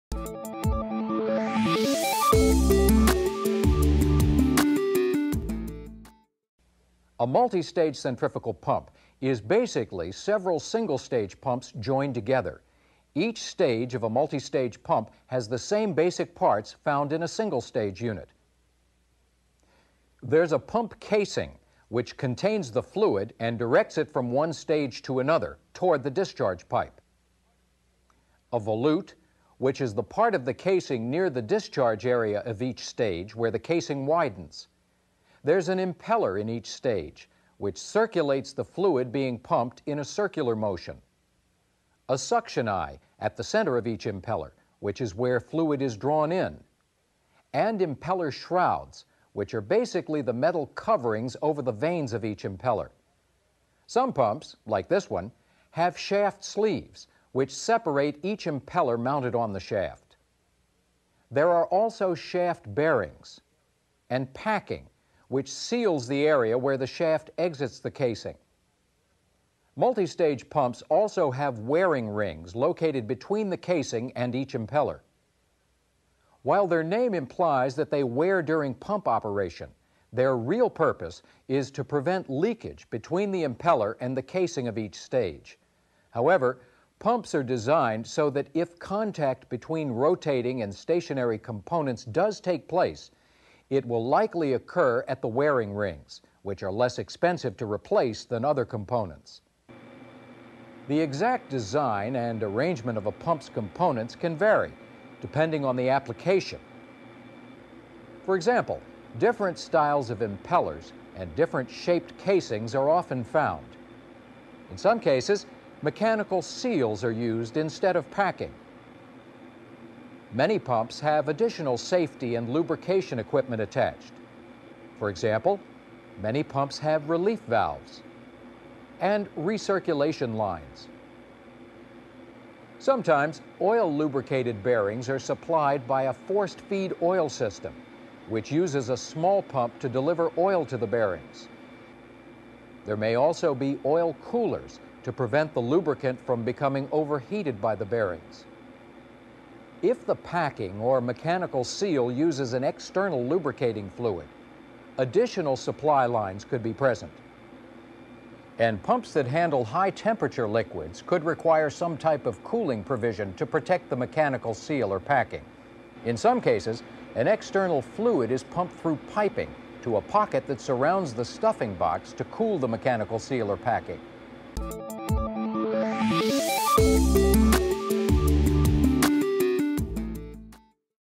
Multistage Centrifugal Pump Maintenance
This course describes the general operation of multistage centrifugal pumps and explains how to identify problems with these units. The disassembly and reassembly of two types of multistage centrifugal pumps are also covered.